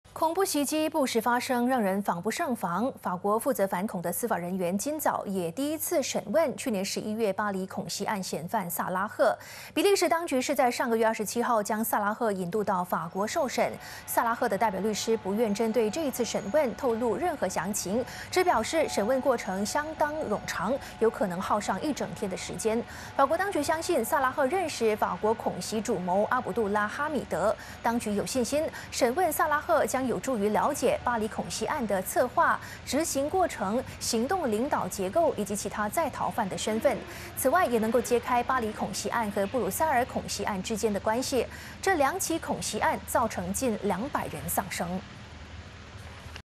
法国今审问巴黎恐袭嫌犯 揭袭击案详情 [20160520]
恐怖袭击不时发生，让人防不胜防。法国负责反恐的司法人员，今早，也第一次审问，去年11月巴黎恐袭案嫌犯萨拉赫。